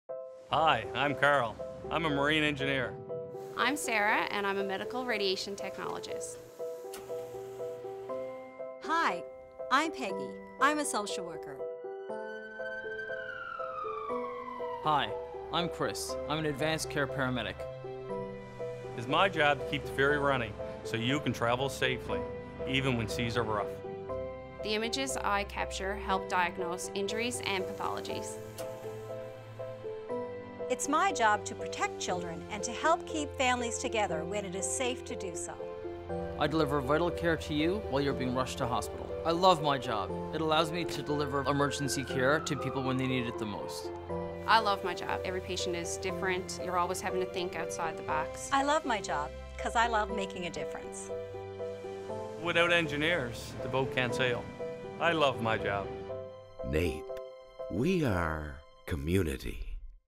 NAPE - We are Community 1
Many people in Newfoundland and Labrador know who NAPE is, but don’t fully understand who makes up the union and the full scope of the vital work NAPE members provide across the province.

During these difficult times in the province, we wanted to shine a light on the hardworking and dedicated members of NAPE that deliver high quality and vital goods and services to the people of this province.

The ‘NAPE – We are Community’ campaign will be a longer term campaign that will focus on the real people behind the jobs, behind the services, behind the statistics.

The campaign will show real NAPE members in their workplaces, talking about their jobs and the impact they have in their communities.